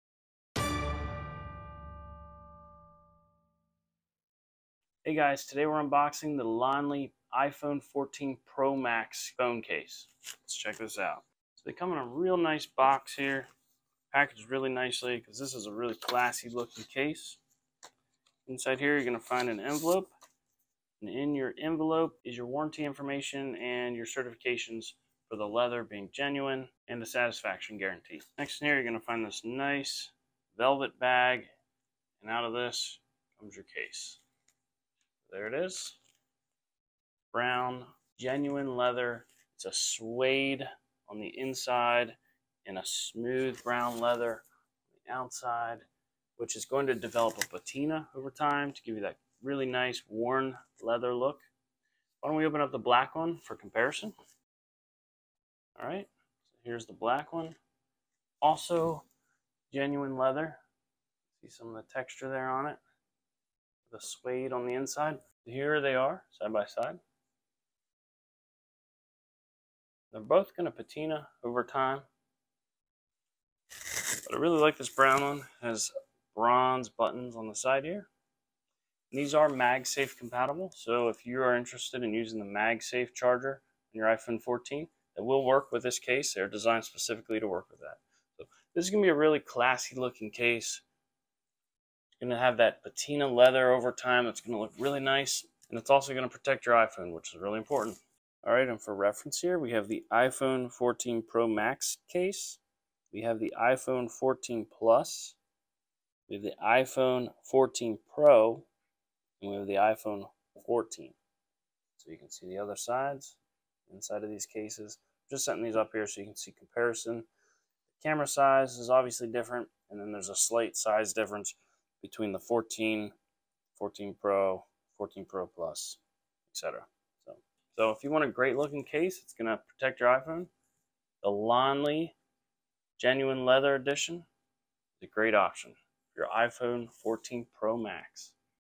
LONLI iPhone 14 PRO MAX Case - Unboxing and Review
This is a great looking case.

LONLI Edition - (for iPhone 14 Pro Max) - Premium European Genuine Leather Case | Develop Patina Over Time | Compatible with Magsafe - Brown